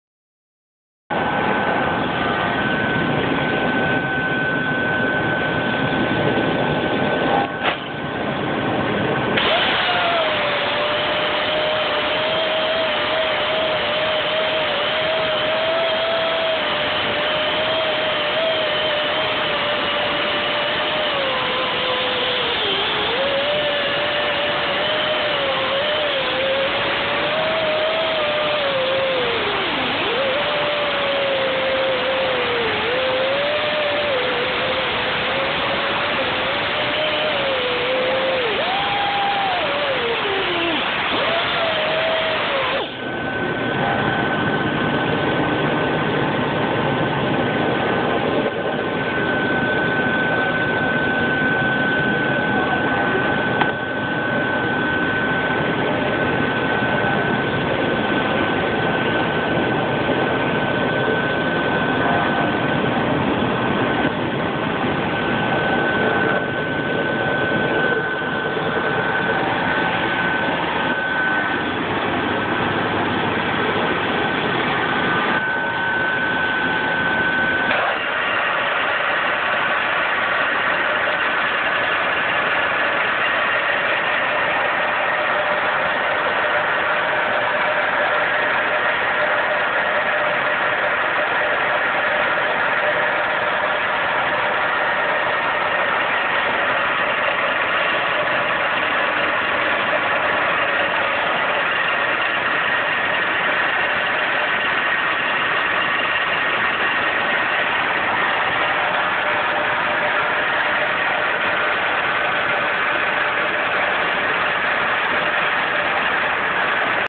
Designed and built a Water & Oil drilling system that has a easy and quick efficient system
Designed and built a Water & Oil drilling system that has a easy and quick efficient system that can be used on smaller scale